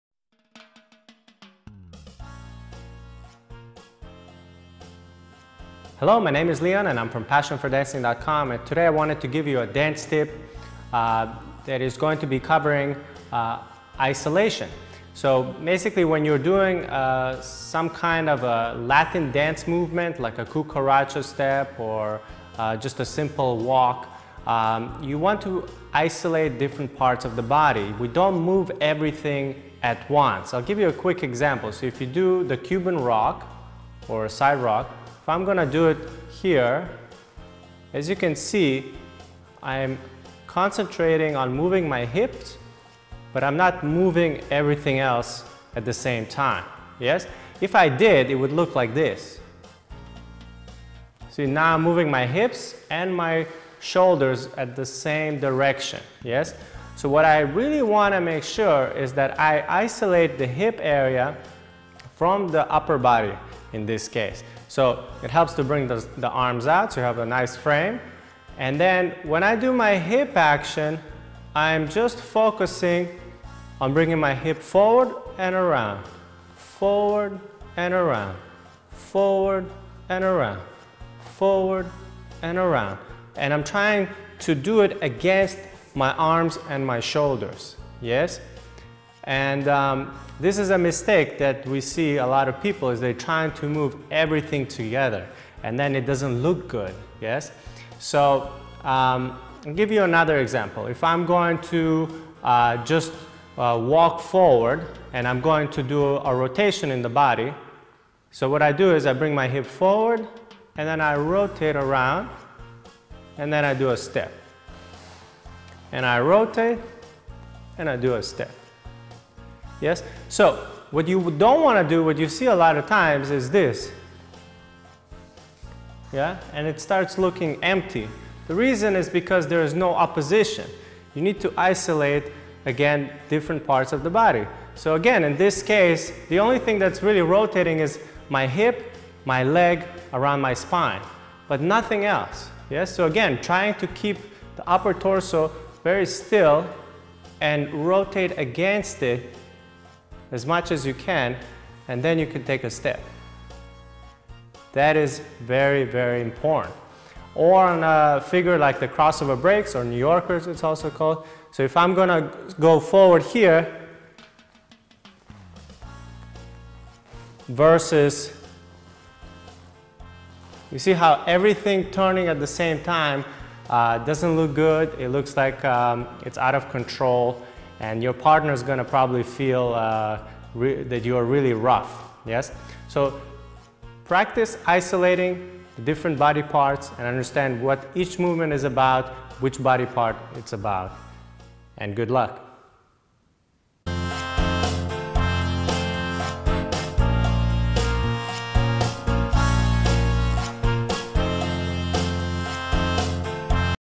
Isolating Body Parts in Latin dancing
Want More Lessons?

Isolating your body is important to looking good on the dance floor.